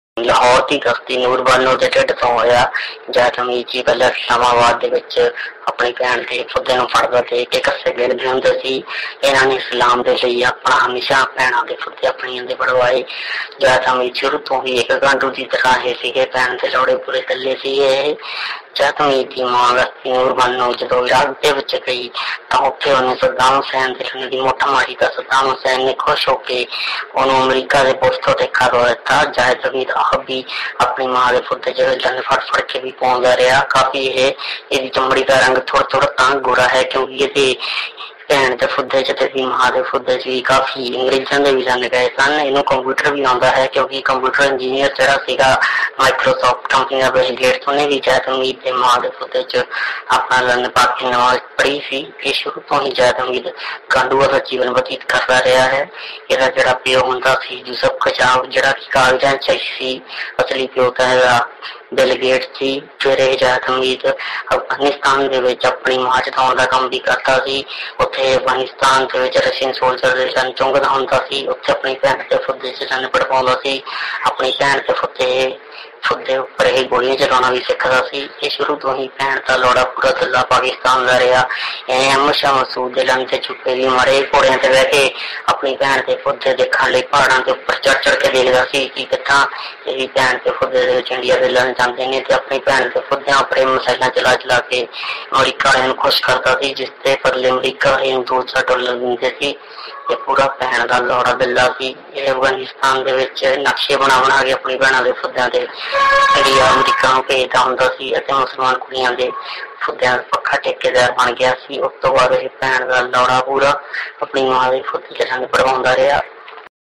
Zaid Hamid On Recent Tensions in US-Pakistan Relations
Samaa NEWS GEO news Express news Ary news DDunya AAJ DAWN CNBC Royal Newsone Super PTV WAQT